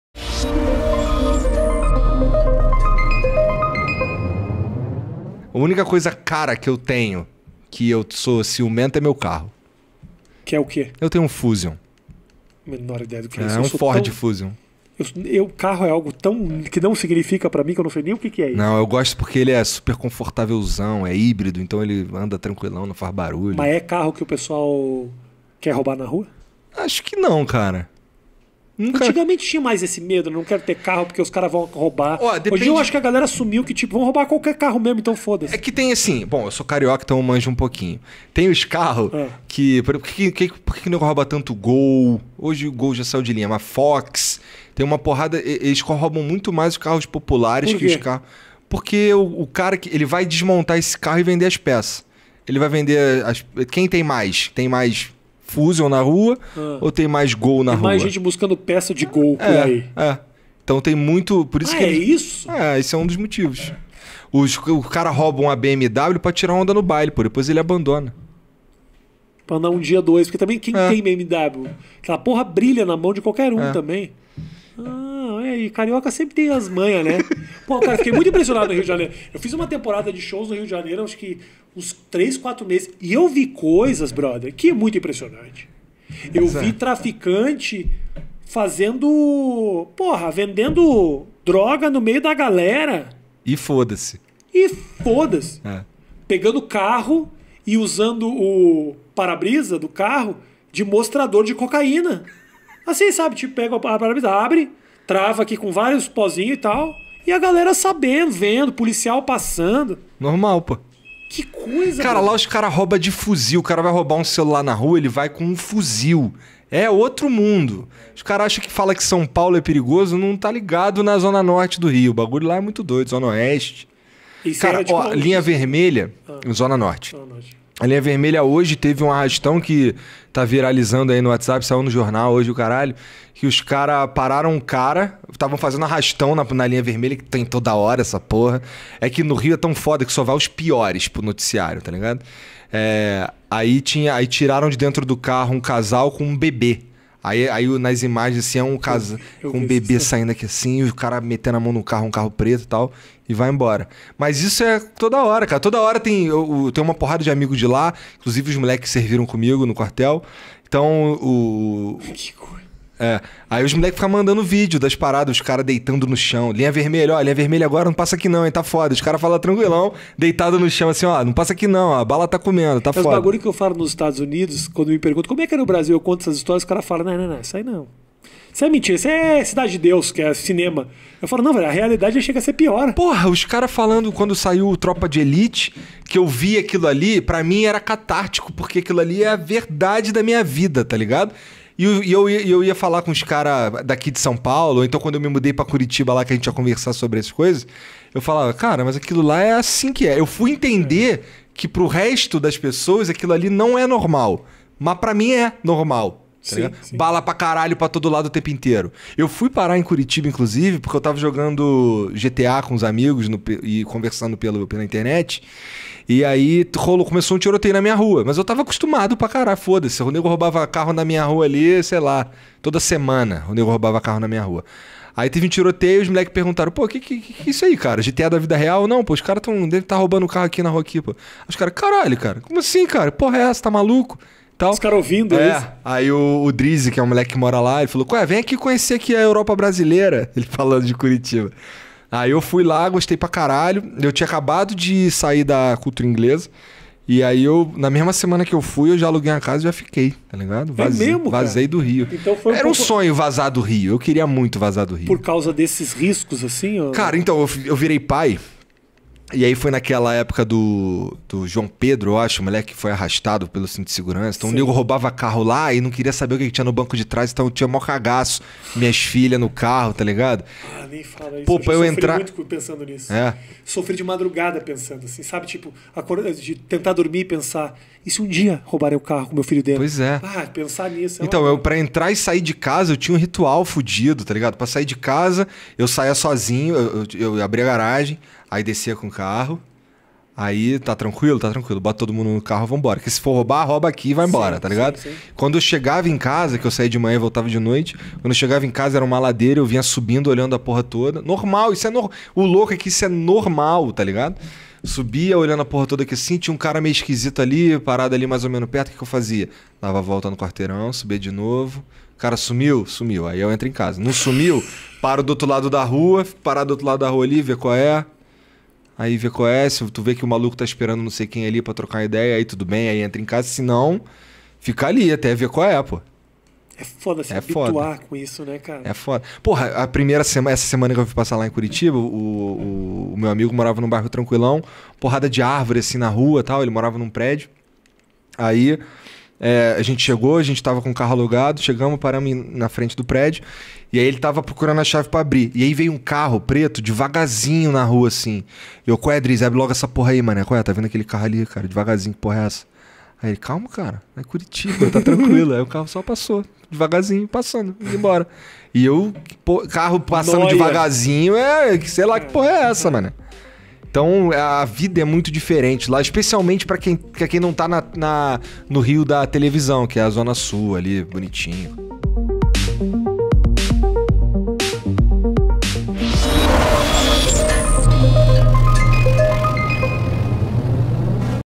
IGOR 3K "DICAS DE COMO VIVER NO RIO DE JANEIRO O INFERNO NA TERRA"! | RAFINHA BASTOS - PODCAST TV
O conteúdo apresentado neste vídeo, é de total direto do canal RAFINHA BASTOS: